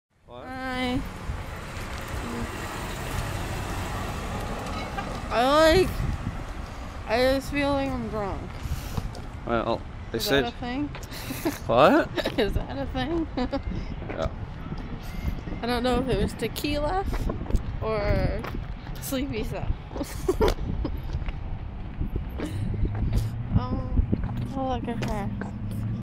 Emily Outdoors
After her Wisdom teeth surgery  June 10, 2015